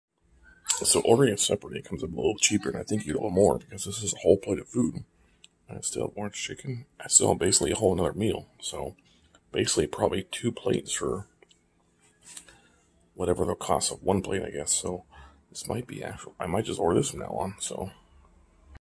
more for less panda express 🤔
this time I ordered a large orange chicken and small rice ...it came out to less that a bigger plate and I have 2 meals ...I'll just order it like this when I do it next 🤷